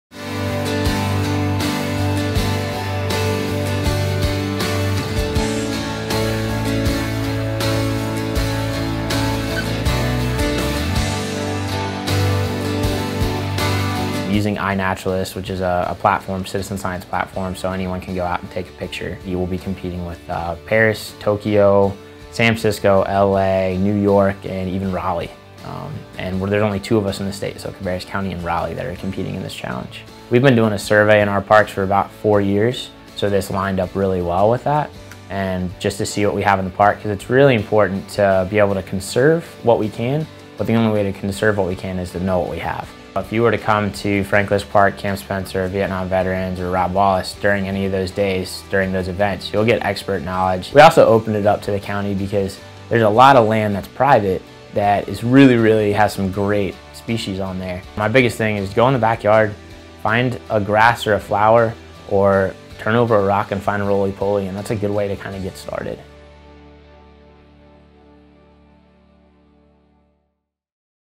Cabarrus County hosts 2018 City Nature Challenge
Cabarrus County is one of two North Carolina communities taking part in the global City Nature Challenge, which calls for folks to get out, discover and document the plants and animals in the environment.

Learn more in this short video about the challenge, which is a joint effort of Cabarrus County Active Living and Parks, City of Concord Government, NC Wildlife Federation and Concord Wildlife Alliance.

Visit @CabCoCNC on Facebook to get more information.